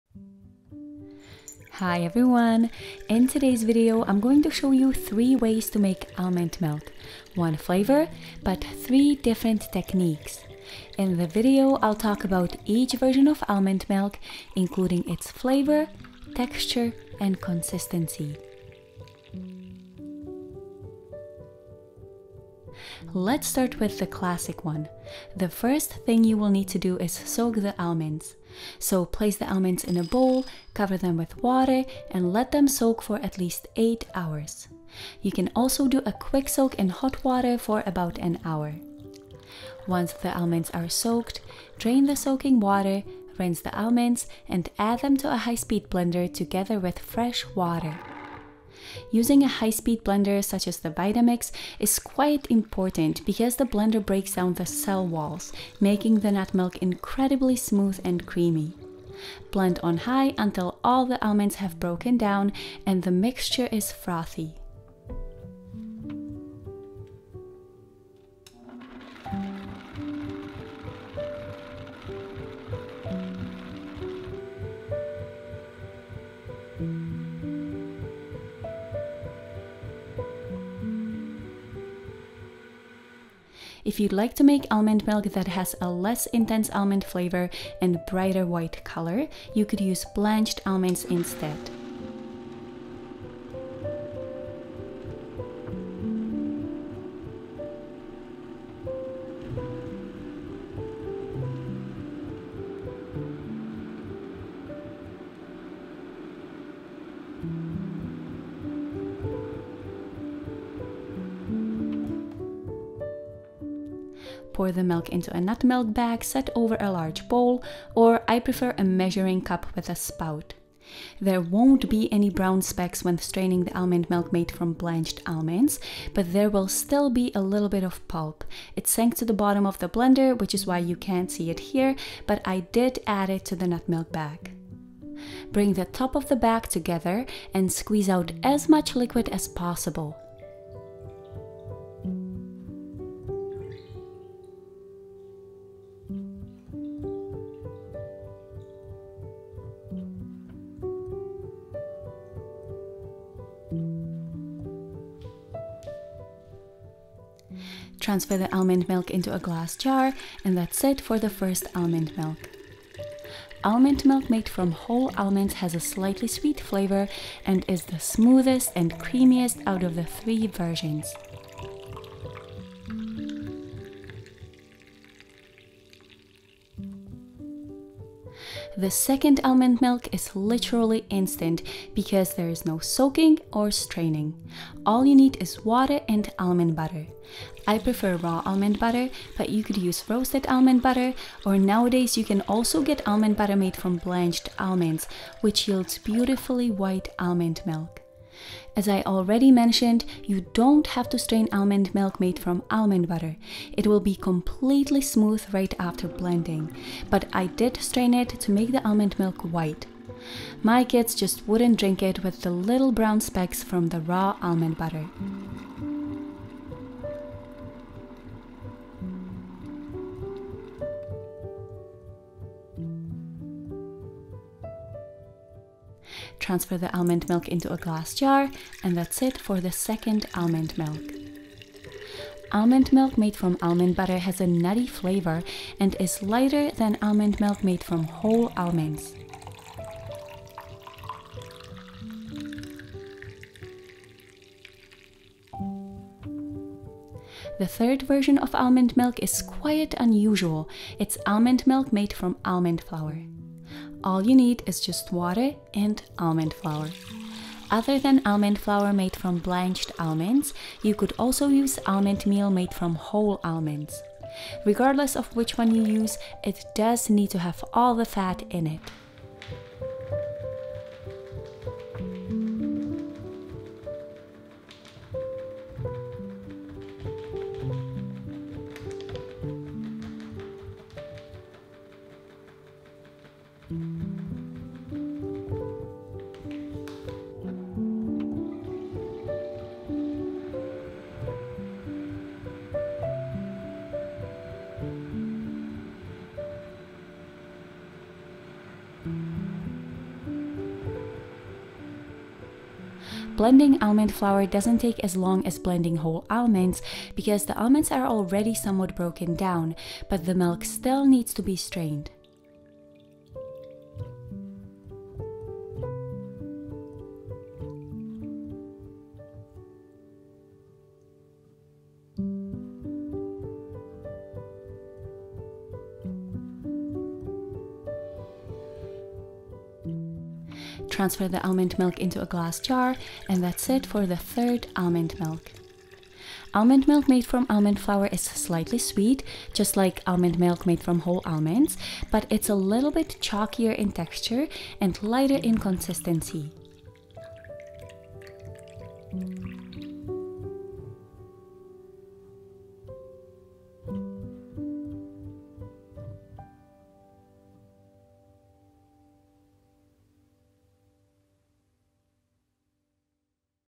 3 Ways to Make Almond Milk - Classic, Instant, and Cheat Version (Elmhurst Inspired)
GET A FREE RAW DESSERT E-COOKBOOK with video tutorials and taste tests

Making homemade almond milk is so simple and affordable that it’s really unnecessary to buy it in the grocery store. It’s worth trying to make at home, at least once, so you understand the difference between homemade almond milk and shelf-stable almond milk from a carton. When you make almond milk at home, stripped down to its bare essentials (nuts and water), it becomes more than just almond water.